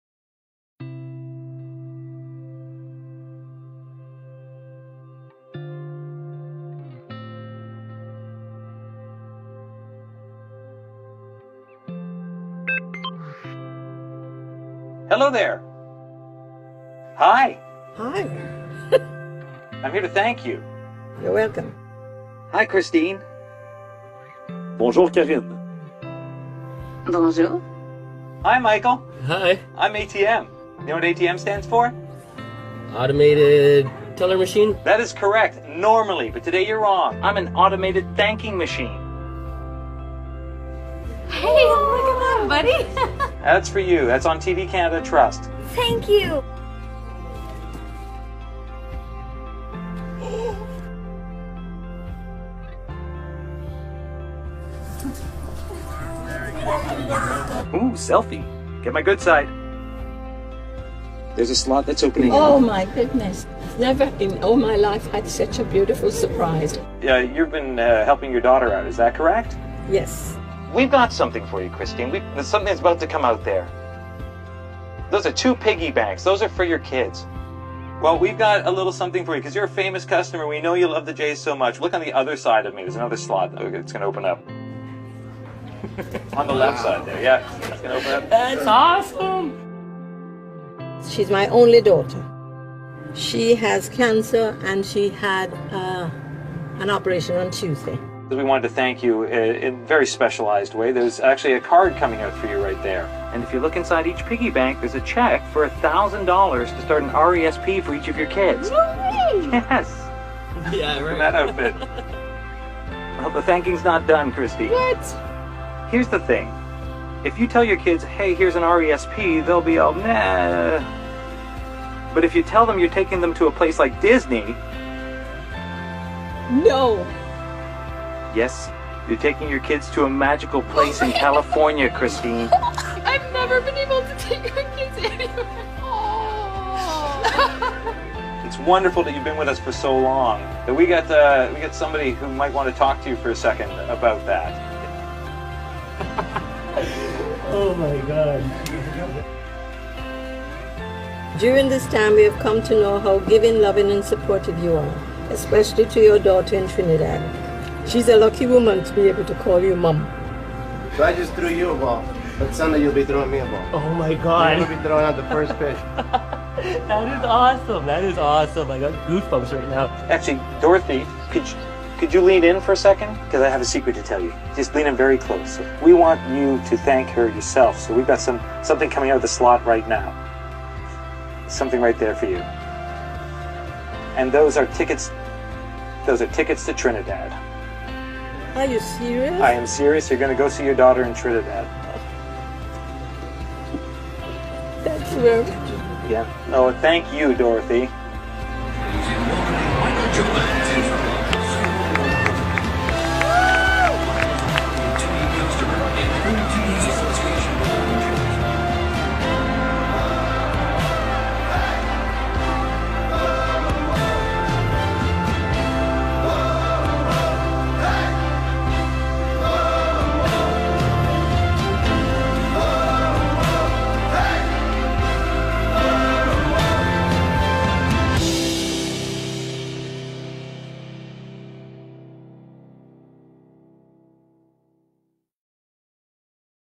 How to use ATM | Special One
This ATM is so Generous. It disburse Cash as well as emotions and lots of other things.
For a little shopping visit Agha's online store.